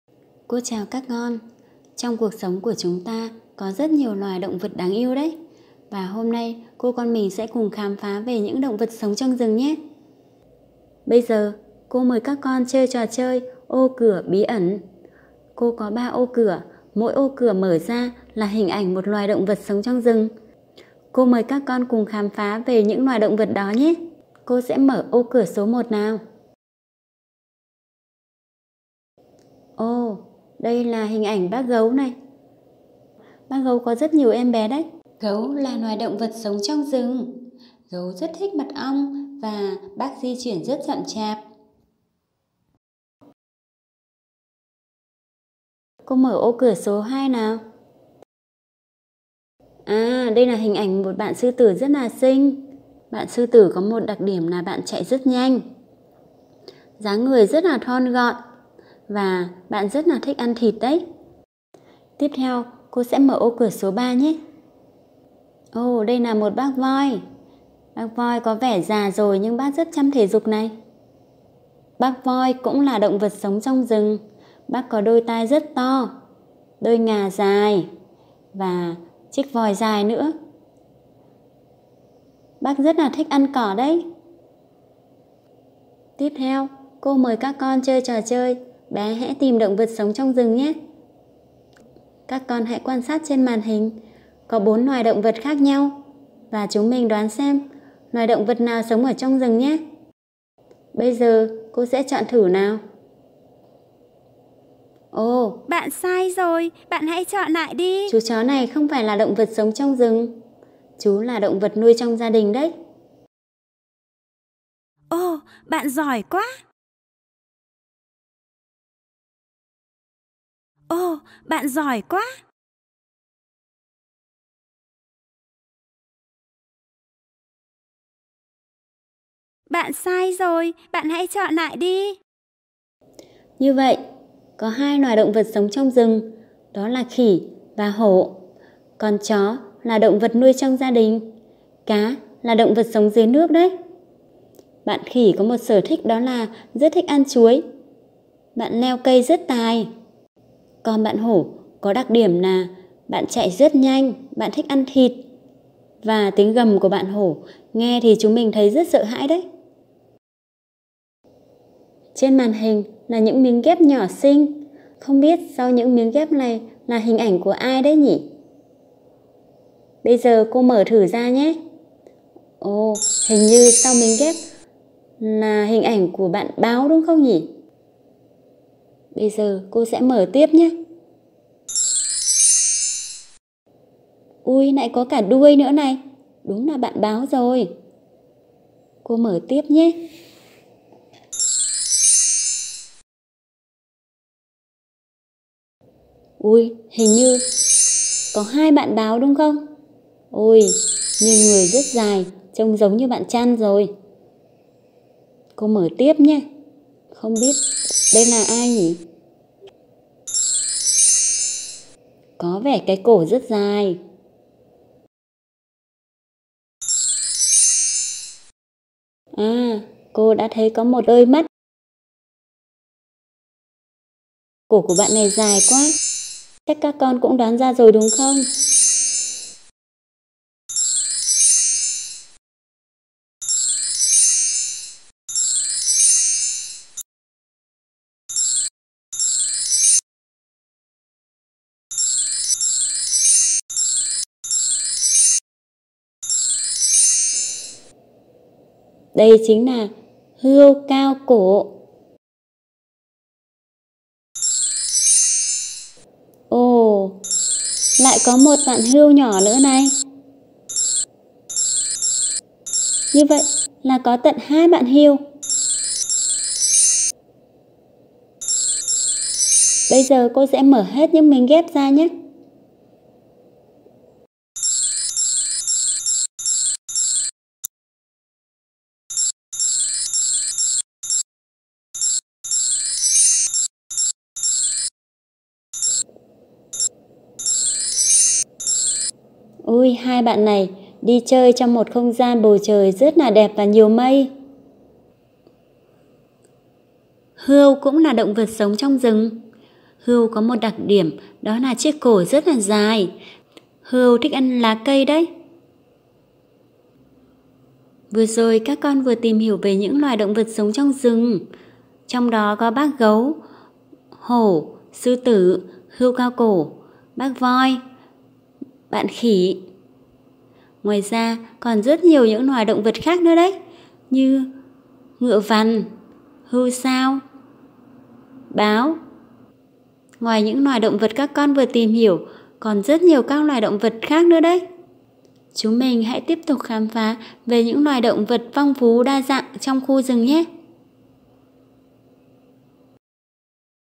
Bé học online - Hoạt Động Khám Phá: Động vật sống trong rừng
Đây là trò chơi bản quyền Edumedia.vn được soạn bằng phần mềm PowerPoint